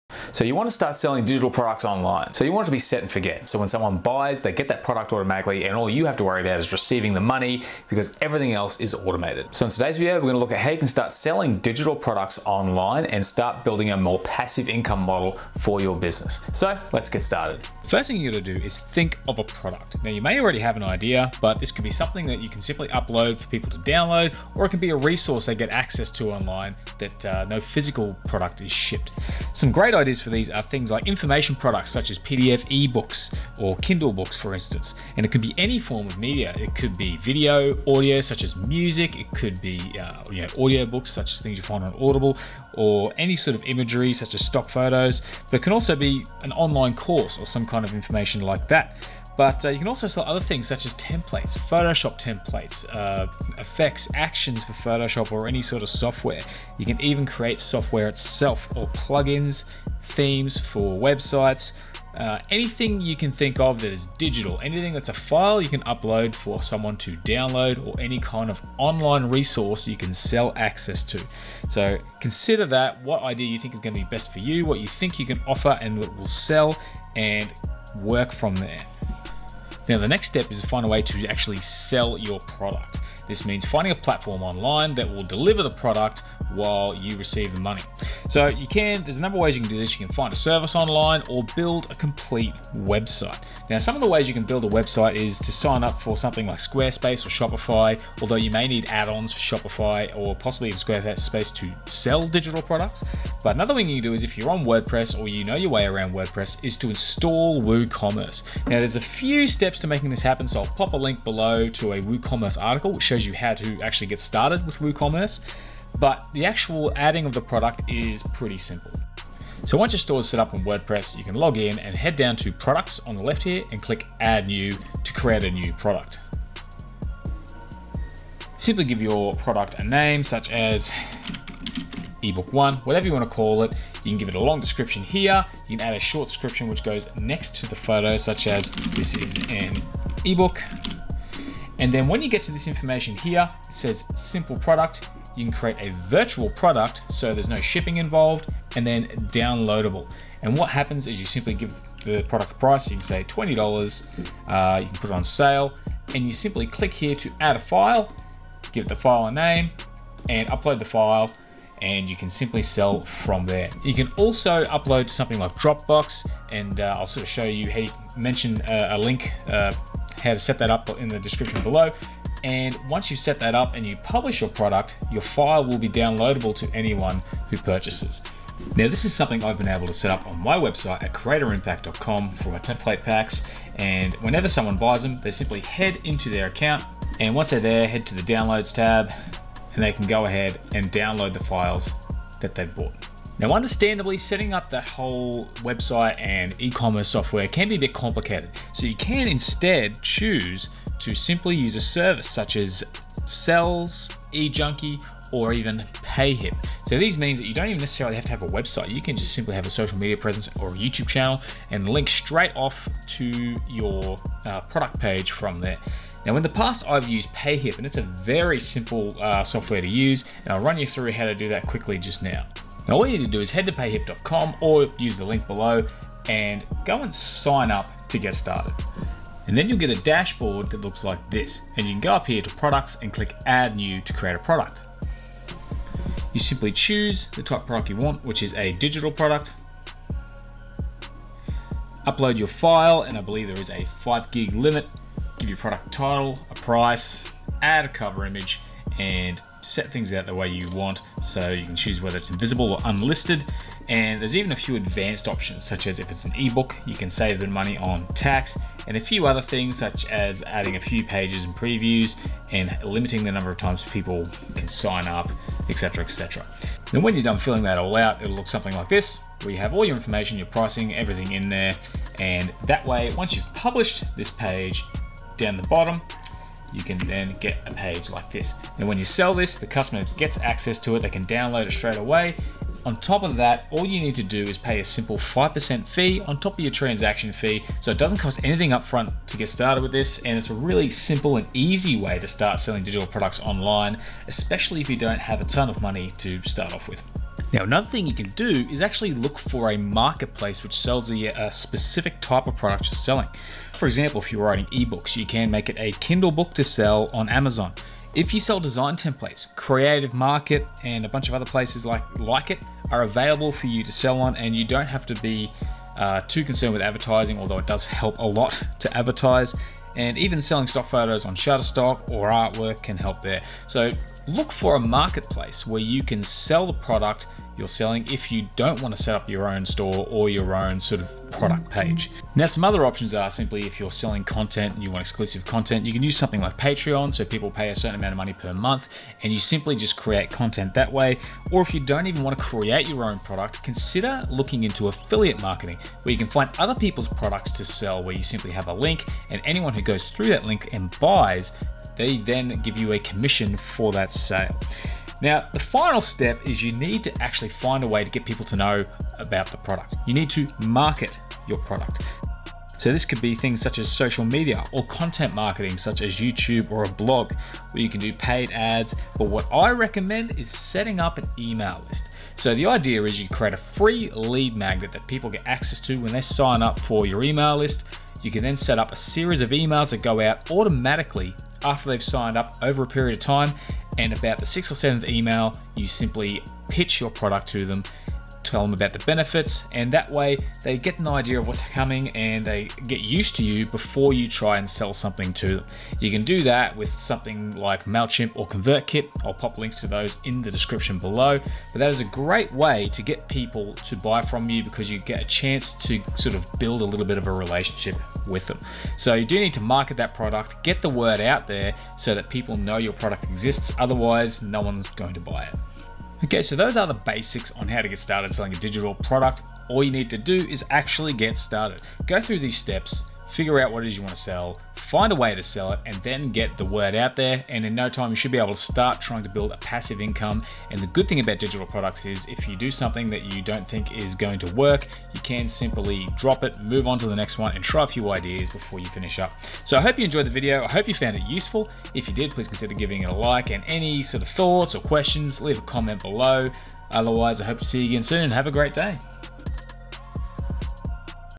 How to Sell Digital Products Online (Ideas & Platforms for 2021 & 2022)
Selling digital products online has never been easier, with a number of platforms to help, you can start building a Passive Income in 2021/2022.  In this video, I cover how to Sell Digital Products Online with some ideas and ways to make it happen.

Starting an online business can be a great way to gain more control over your lifestyle and even scale and existing business.

You can go into things fully invest with a website and learn how to sell digital products on WordPress with Woocommerce, or you could find a quick, instant solution and learn how to sell in payhip. All youneed are some solid digital product ideas to start.

You can sell courses online, ebooks, music, templates or anything quite easily.  It's not overly difficult to come up with ideas for digital products and start a digital business.  There is no best platform to sell digital products, but a solid handful that can help.

Make the start today and work towards a more flexible income and lifestyle.

// RESOURCES

WordPress (recommended), using WooCommerce - To start check out the link below (official tutorial):

Learn how to get your business or blog online, reaching more people and making more money!

// CREATOR IMPACT